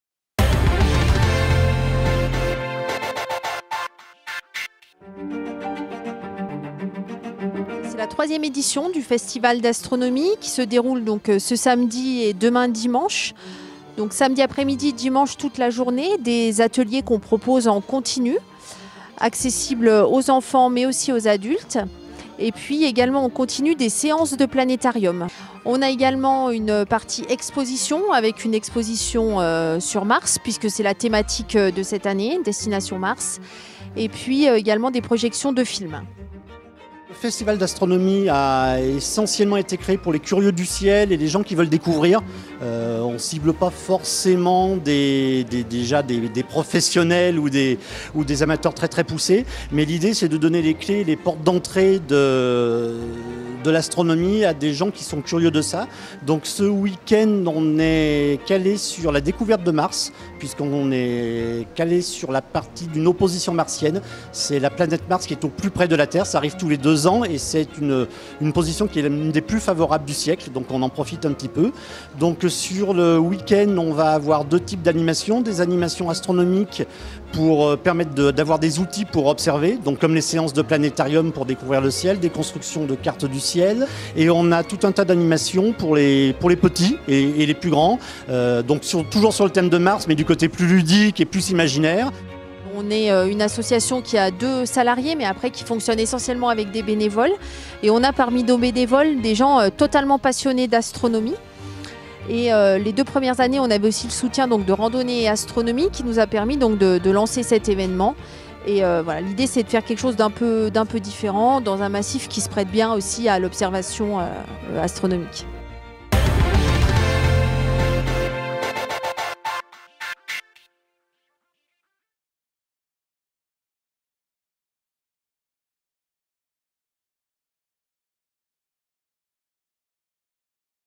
3e festival d'astronomie aux Aillons Margériaz
Les Aillons-Margériaz organisent le 3ème Festival d'Astronomie. Au programme : exposition, planétarium avec vue sur les planètes à 360°, projection de film, animations ludique sur Mars pour les enfants, tir de fusée prévue ce dimanche après-midi.
Venez découvrir toutes les animations pour les grands et petits, toute la journée, au centre d'accueil d'Aillons le Jeune.